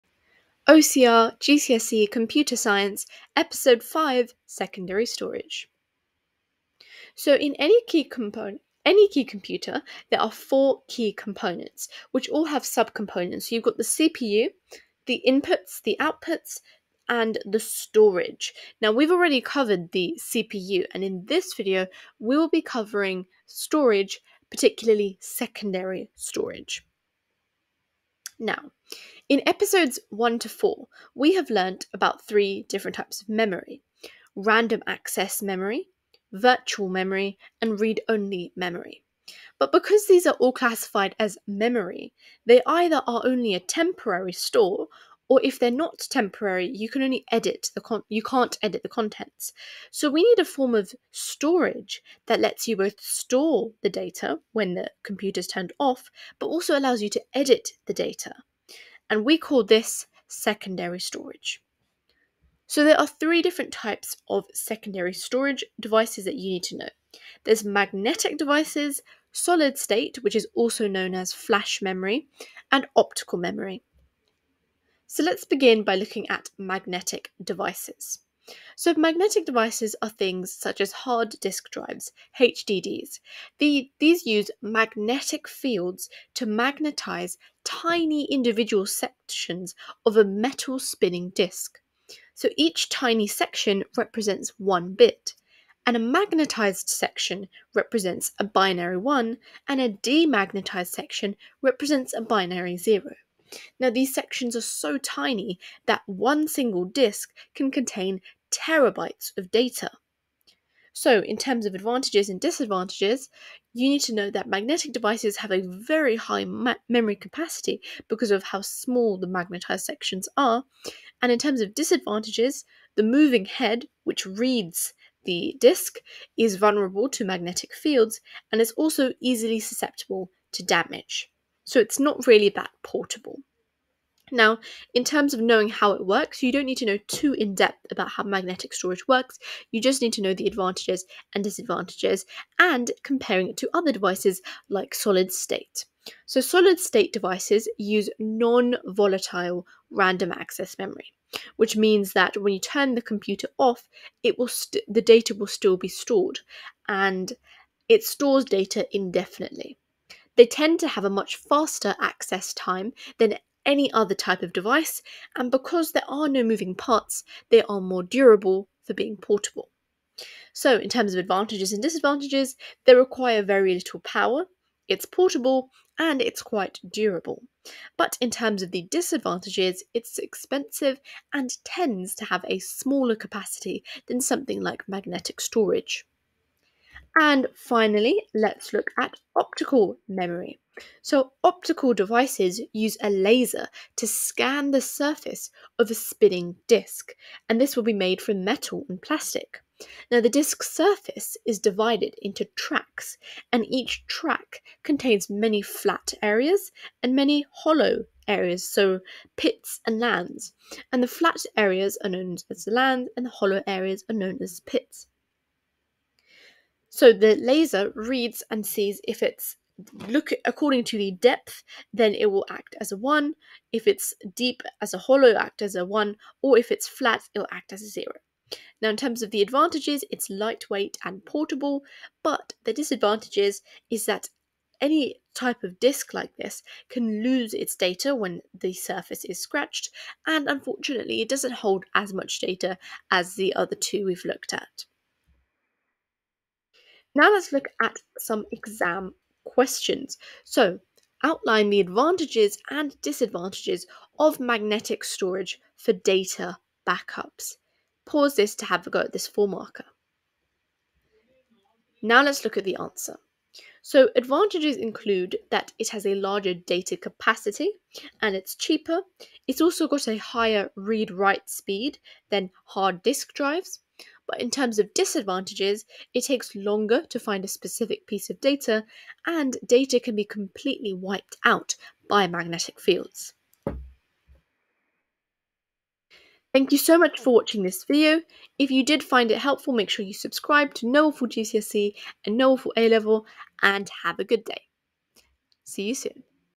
GCSE OCR CS: Secondary Storage ( eps5)
Hiya and welcome to the Fifth video in the OCR GCSE COMPUTER SCIENCE PAPER 1 SERIES!
TIMESTAMPS:
0:00 intro
0:07 Learning Objectives
0:27 Why is secondary storage needed?
1:04  Types of secondary storage
1:15 Magnetic devices
2:22 Solid state devices
3:07 Optical memory
4:06 Exam question practise!